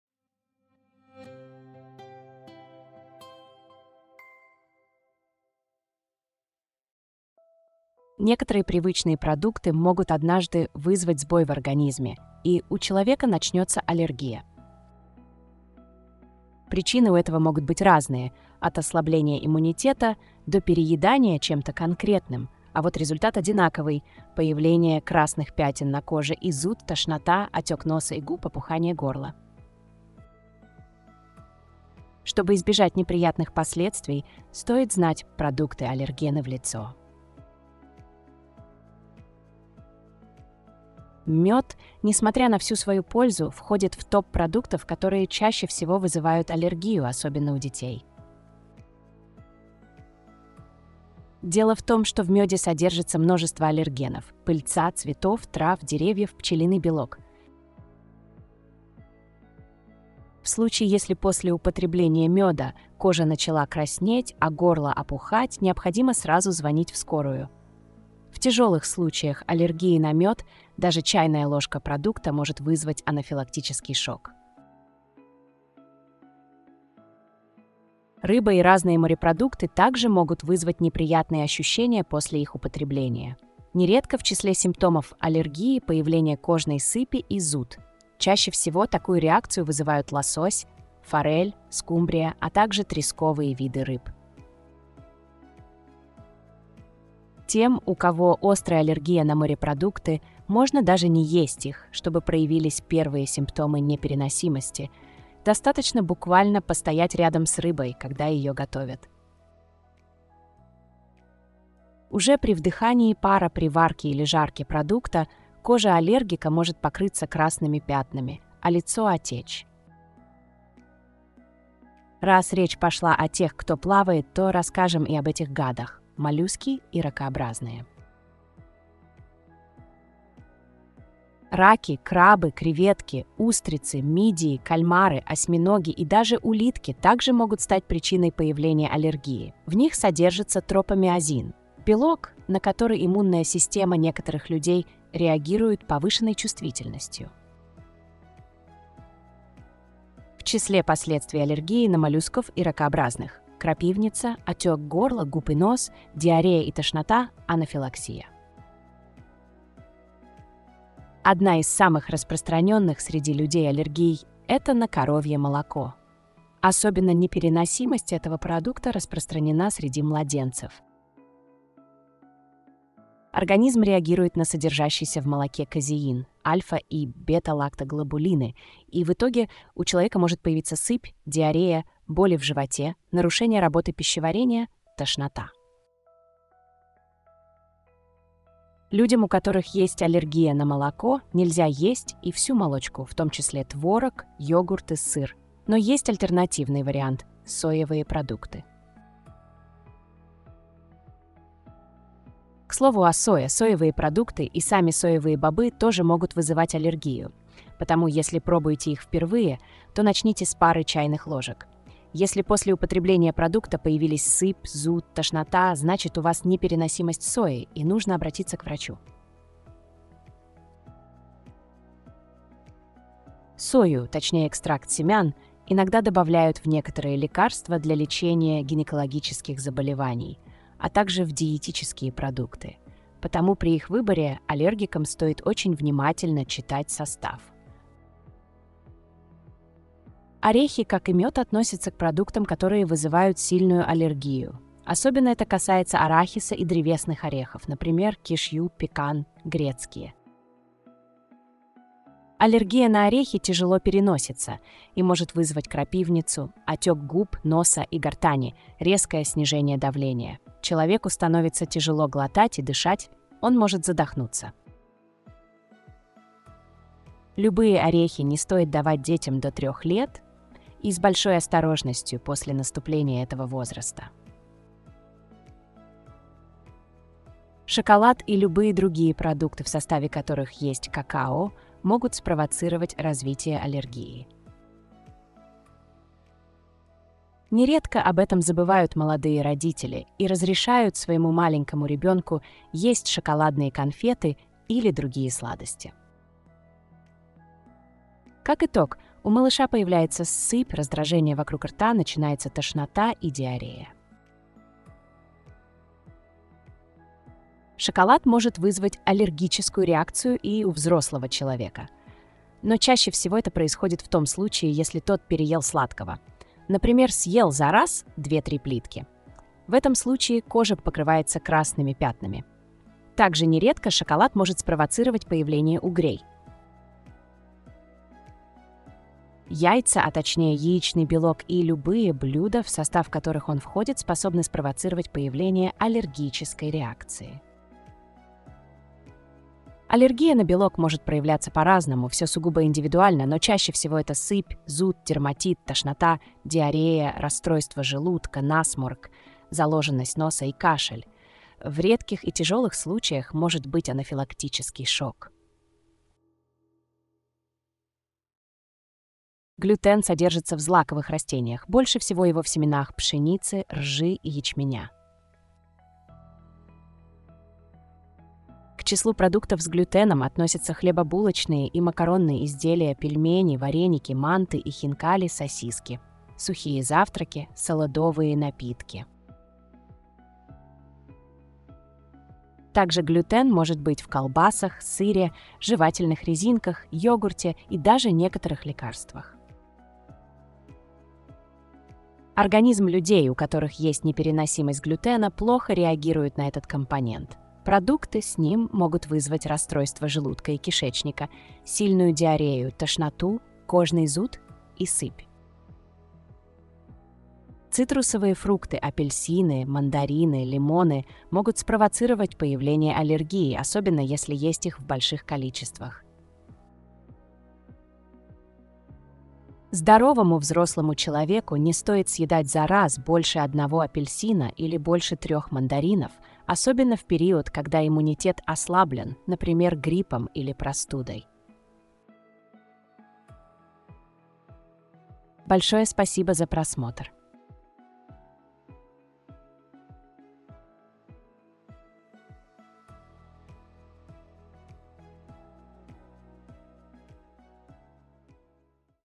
10 самых сильных пищевых аллергенов на нашем столе
Некоторые  привычные продукты могут однажды вызвать сбой в организме, и у человека начнется аллергия.

Some familiar products can one day cause a malfunction in the body, and a person will develop an allergy.